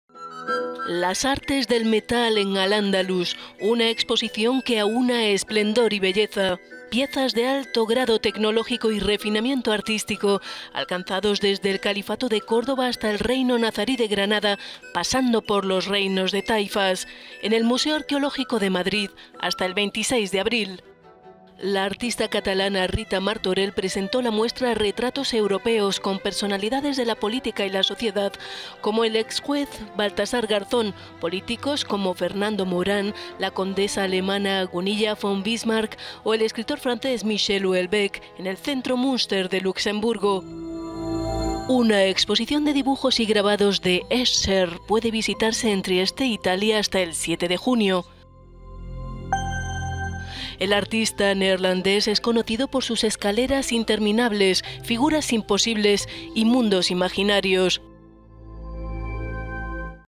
Rendez-vous: La agenda cultural europea
Un recorrido por Madrid, Trieste o Luxemburgo, tras las exposiciones más fascinantes de este invierno.…